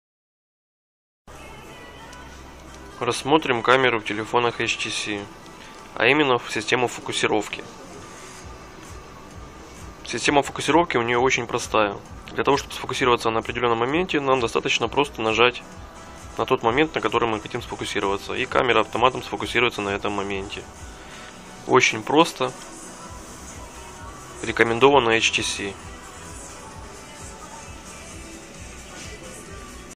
Камера и система фокусировки.
Камера в телефонах НТС и ее возможность фокусировки на определенном фрагменте.